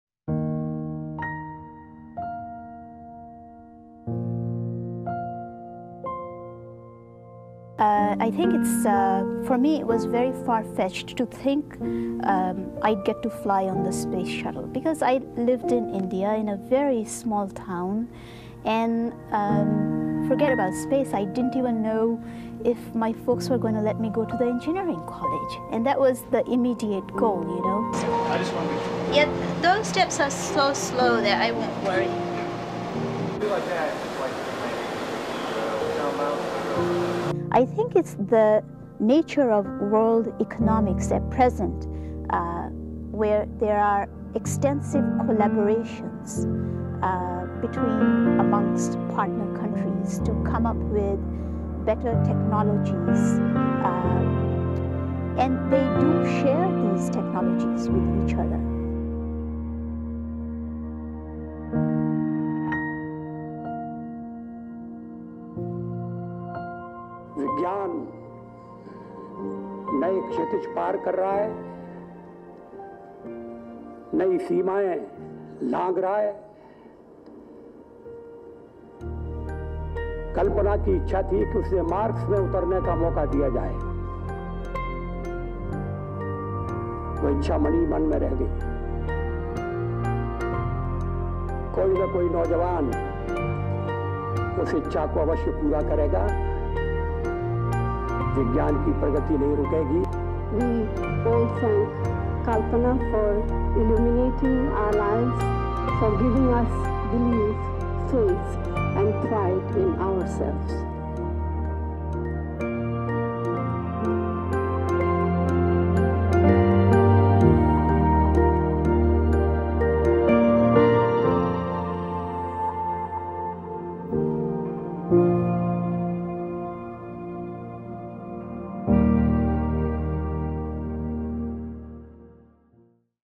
Kalpana Chawla | First woman of Indian origin in space
Kalpana Chawla (March 17, 1962 – February 1, 2003) was an American astronaut and the first female of Indian origin to go to space.